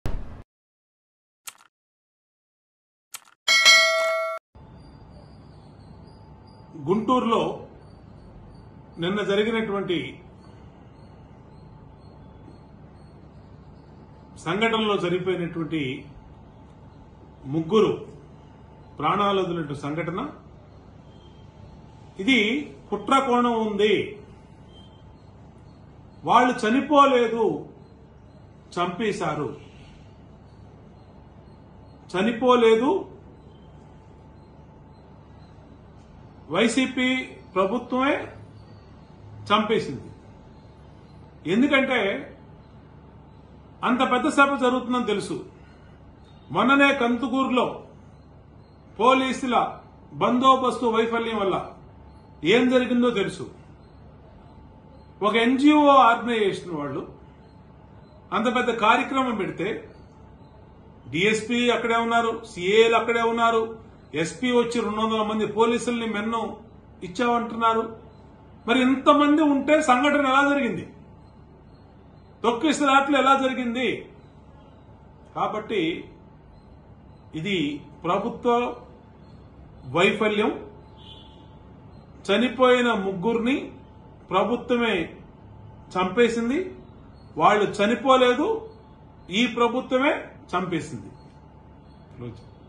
చనిపోలేదు చంపేశారు.. వైసీపీ పై, టిడిపి సంచలన ఆరోపణలు... | YSRCP | TDP | Bonda Uma
చనిపోలేదు చంపేశారు.. వైసీపీ పై, టిడిపి సంచలన ఆరోపణలు...